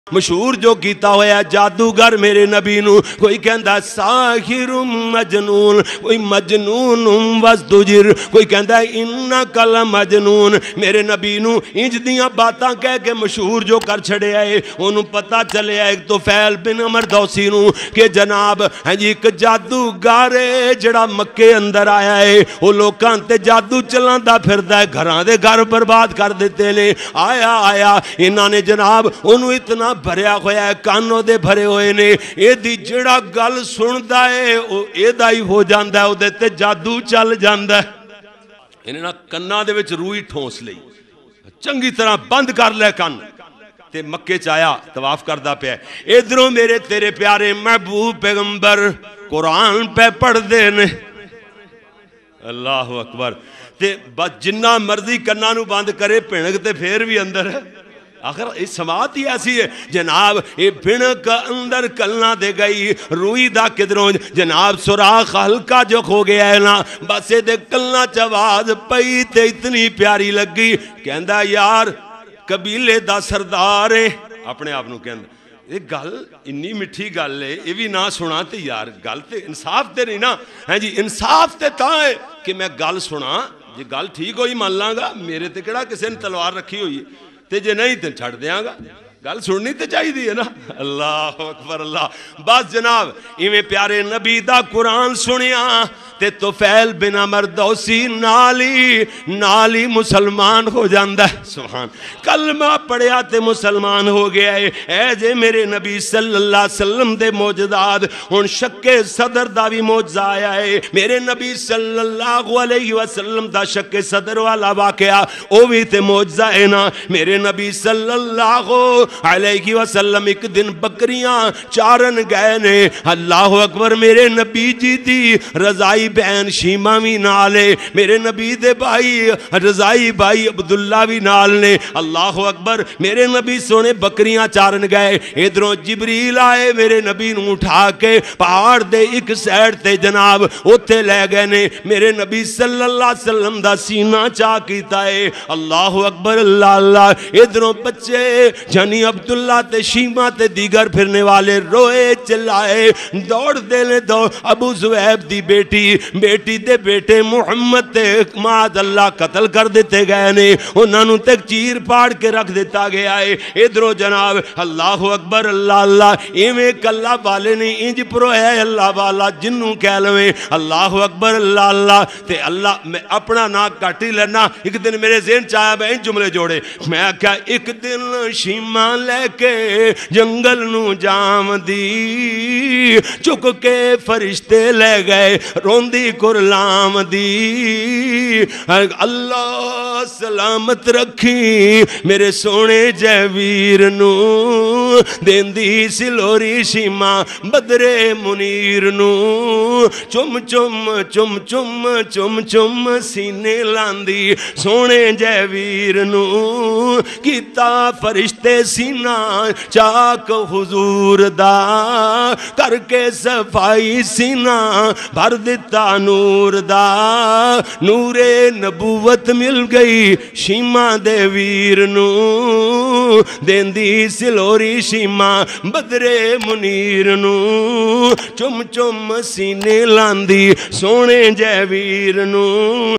Mujazat Rasool Qari Yaqoob Faisalabadi Ramzan 2023 Clip # 14
نبی صلی اللہ علیہ وسلم کا سینہ چیر دیا کس نے اور کیوں
قاری محمد یعقوب صاحب فیصل آبادی